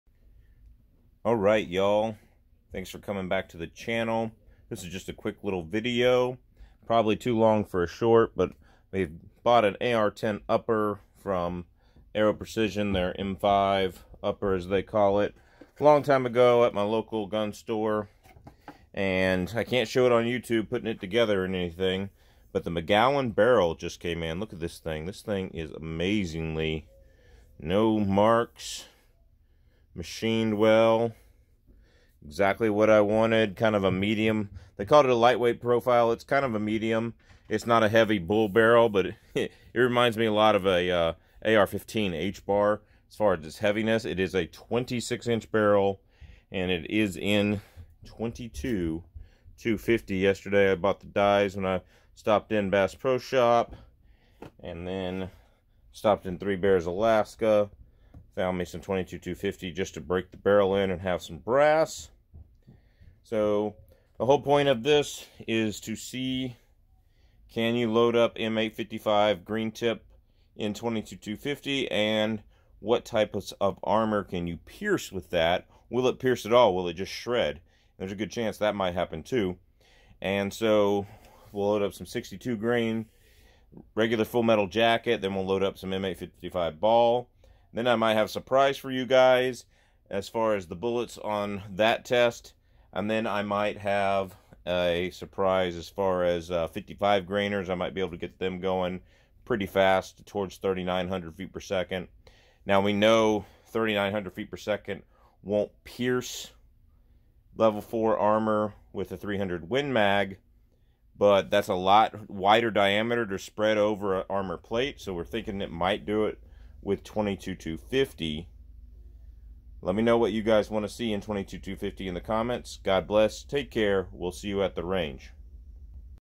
.22-250 AR-10 Project PREVIEW VIDEO
Just a little discussion about my .22-250 project Starting with the McGowan Barrel.

Shall Not Comply! Now Tri-Star Trading!